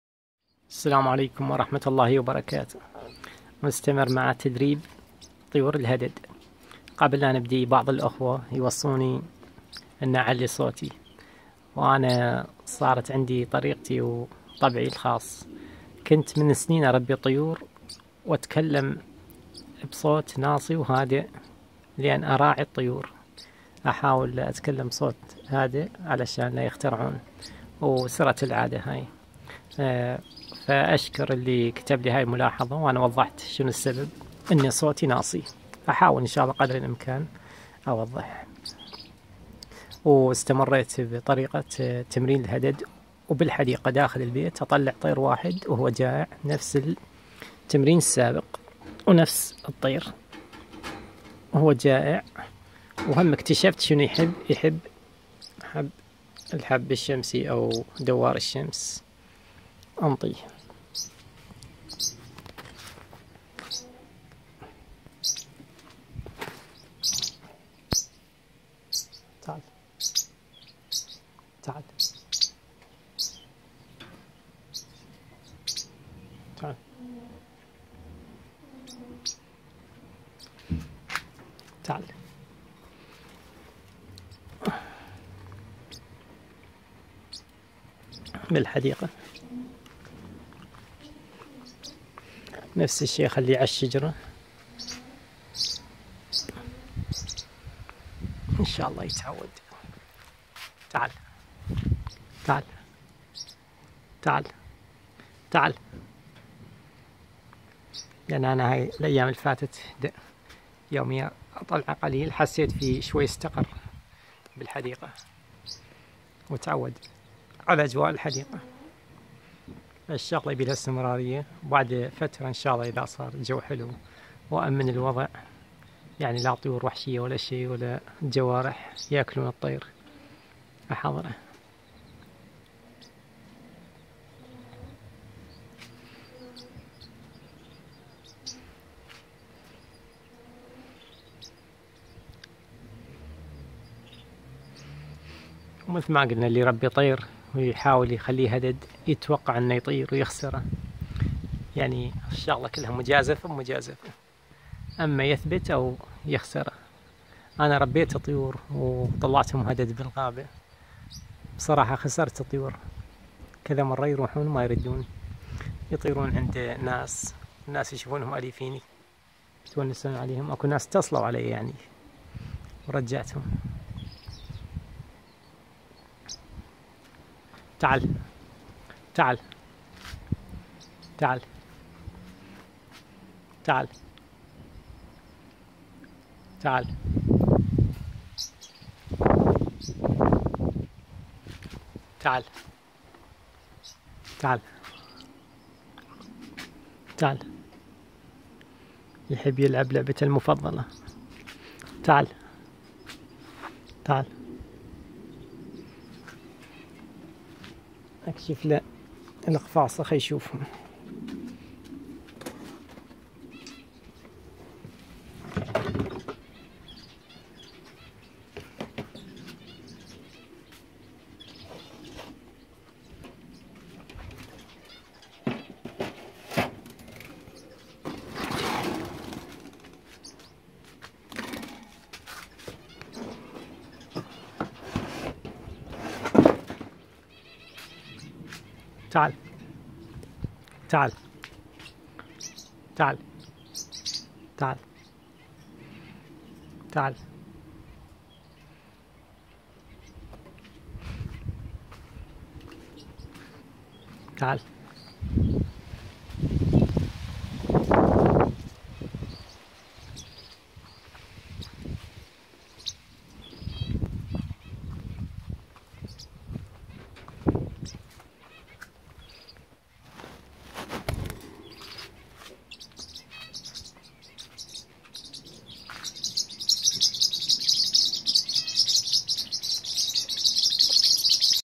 طيور هدد 2
التعويد على  أجواء  الحديقة